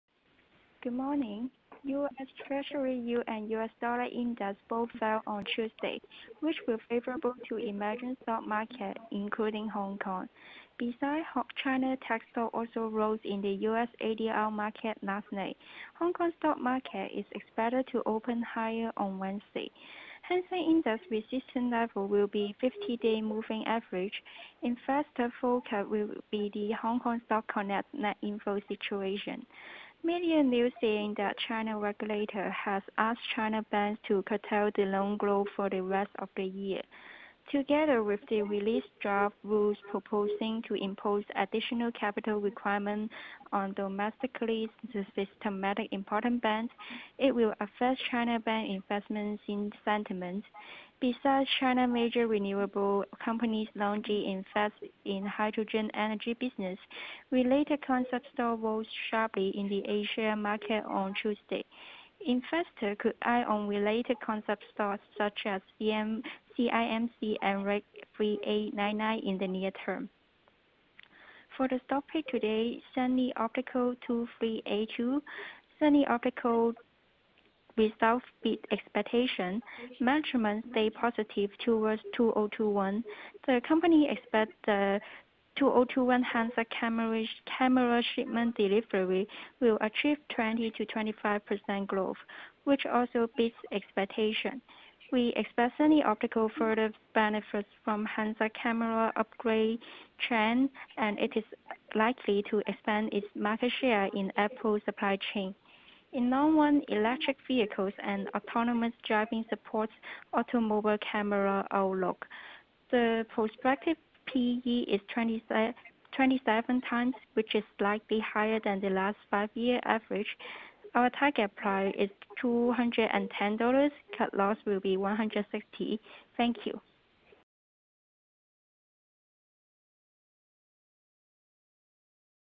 Resistance at 50 Day SMA; Eye on Southbound Trade Net Inflow
KGI Morning Briefing-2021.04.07 SunnyOptical (2382)
Stock Pick
Sunny Optical(2382): Sunny Optical(2382) FY20 result beat expectation. Management stay positive towards FY21. The company expects that FY21 handset camera shipment delivery achieves 20-25% growth, which also beats expectation. We expect Sunny Optical further benefits from handset camera upgrade trend. And it is likely to expand its market share in Apple supply chain. In long run, electric vehicles and autonomous driving supports automobile camera outlook. FY21 forecast P/E is 27x which is slightly higher than last five year average. Target price is HK$210. Cut loss price is HK$160.